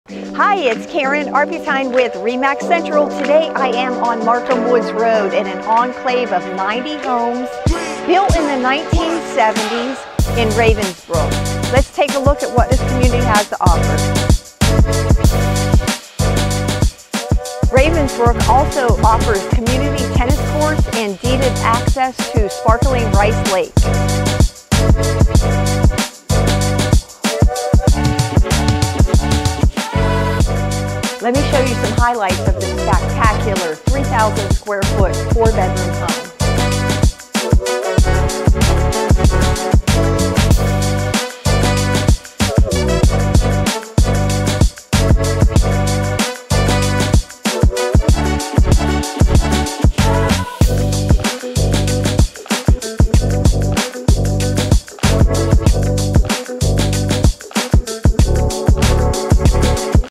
Ravenbrook in Longwood, Markham Wood's Road - "Hidden Gem"
Ravensbrook in Longwood is located northwest of I-4 and a short drive from downtown Orlando is a quiet and desirable non-gated community of single-family homes. The neighborhood features a variety of homes differing in styles starting from approx. 2400 square feet. There are several lakes throughout the area, including Rice Lake which is practically in your backyard! Another benefit of residing in Ravensbrook is Magnolia Plantation Park is just up the road if you need to get outdoors.

Also nearby is the 14 miles long Seminole Wekiva hiking trail, where you can ride a bike or go on a stroll and revel in the panoramic greenery. The trail runs through a west portion of the I-4 trail overpass and is considered part of the Florida National Scenic Trail. Venture out just a little further southwest to observe ample wildlife via canoe or kayak at the Wekiva Springs State Park. If an indoor adventure with the kids is preferred, a short car ride away is Planet Obstacle - the world’s largest indoor obstacle park! Activities range from zip-lining and rock-climbing to a rope course and bumper cars. An onsite café to eat, drink and recharge is also available.

The Alaqua Country Club is in close proximity if you’d like to experience the most exclusive golf club in the North Orlando area. By invitation only, enjoy monthly social events, first-class service, and all the perks a reclaimed and private club has to offer. Topgolf is another nearby sports entertainment complex that’s a short 10-minute ride up the road.

Shoppes of Oakmonte and Lake Mary Centre shopping malls are conveniently situated just northeast of the community. There you will find access to Home Depot, Target, TJ Maxx & HomeGoods, and other retail store options to suit your shopping needs. Plenty of casual dining options are also available to you – including the well-established Don Julio Mexican Kitchen & Tequila Bar that’s all the rave in the area! Create memories with friends and family while indulging in delicious authentic Mexican cuisine.

Afterward, consider spending an afternoon exploring the city’s heritage at the Lake Mary Historical Museum where you can study photographs and archives associated with the history of Lake Mary and the neighboring associations. Speaking of studying ­­– several schools are in the surrounding area, such as Lake Mary High School where your teen can walk the passageway into adulthood or they can kickstart their career at Seminole State College.

South Seminole Hospital and other medical facilities are also in close range for your convenience. Altamonte Springs is further westbound down Markham Woods Road/I-4 if you’d like to explore other avenues of entertainment that are still nearby!